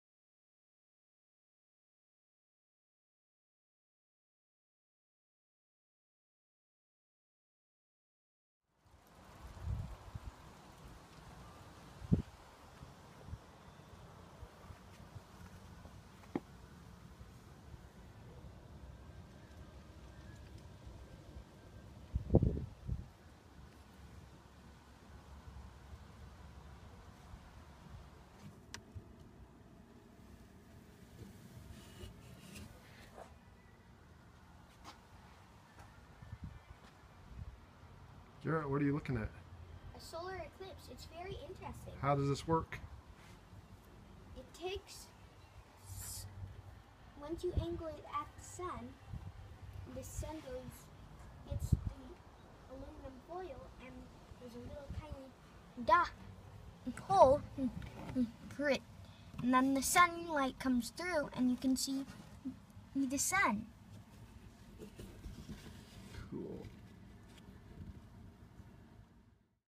Eclipse Viewing 23 Oct. 2014
Partial Solar Eclipse Viewing 23 Oct. 2014, Longmont, Colorado.  We used a Sunspotter telescope and a homemade pinhole viewer to observe the eclipse.  Looking forward to the total eclipse on 21 August 2017!